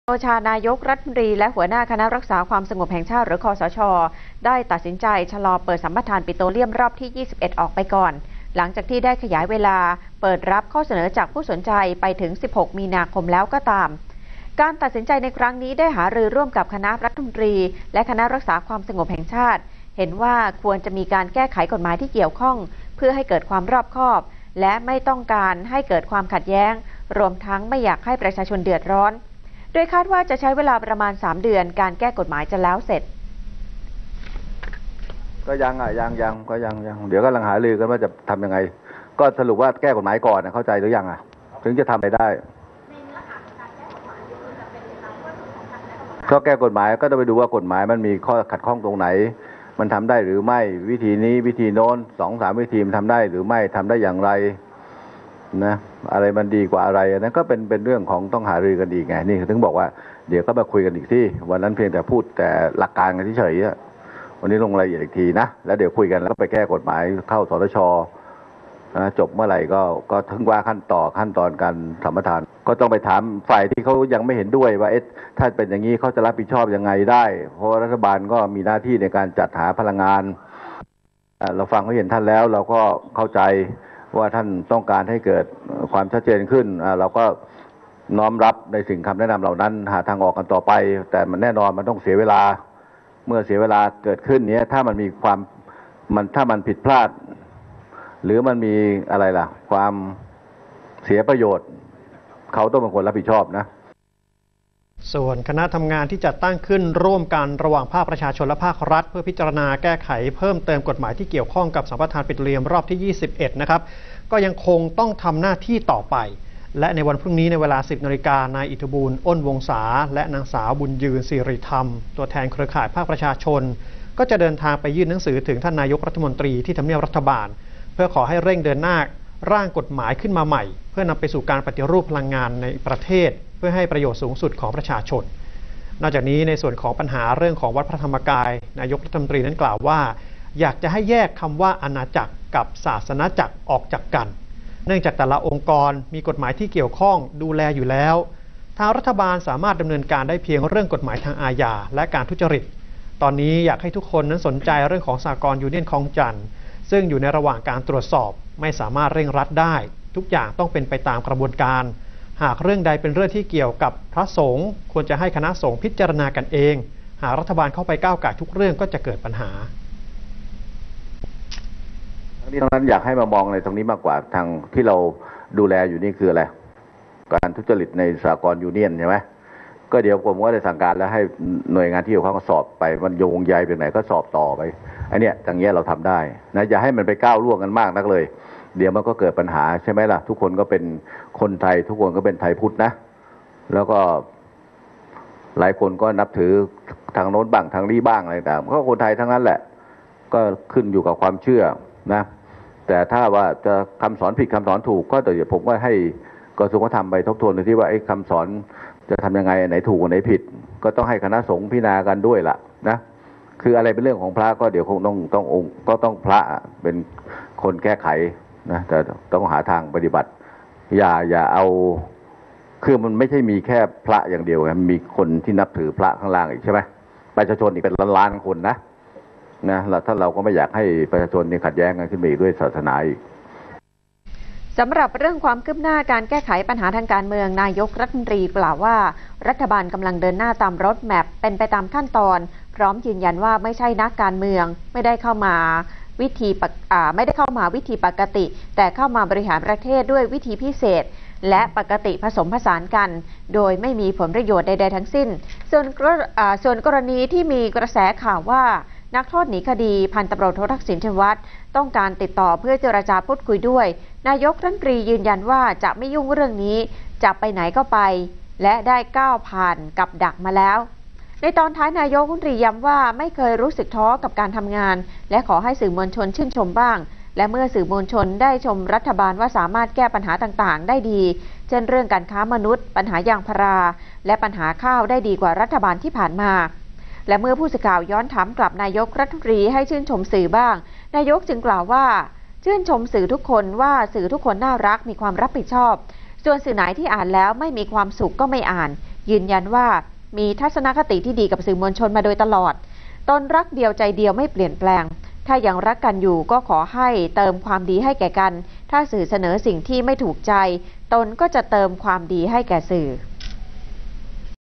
“บิ๊กตู่” ยอมชะลอสัมปทาน สั่ง สนช.แก้ กม.ใน 3 เดือน จี้รับผิดชอบร่วมกัน
2015/02/24 “บิ๊กตู่” ยอมชะลอสัมปทาน สั่ง สนช.แก้ กม.ใน 3 เดือน จี้รับผิดชอบร่วมกัน